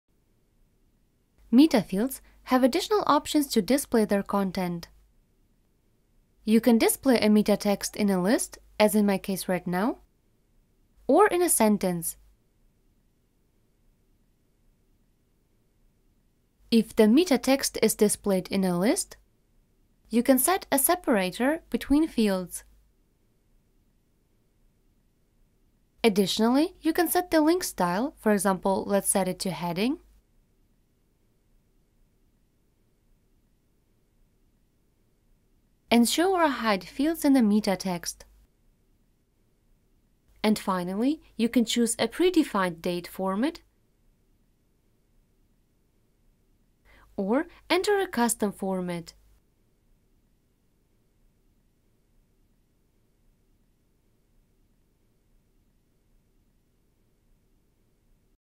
Using the Meta Field Options | YOOtheme Documentation (Joomla)
Meta fields have additional options to display their content.